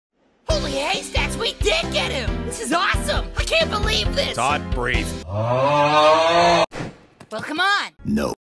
Gen Z Humor Be Like…
Yes, humor has evolved so much that only one single sound effect can make a person laugh, that’s just wonderful I have to say.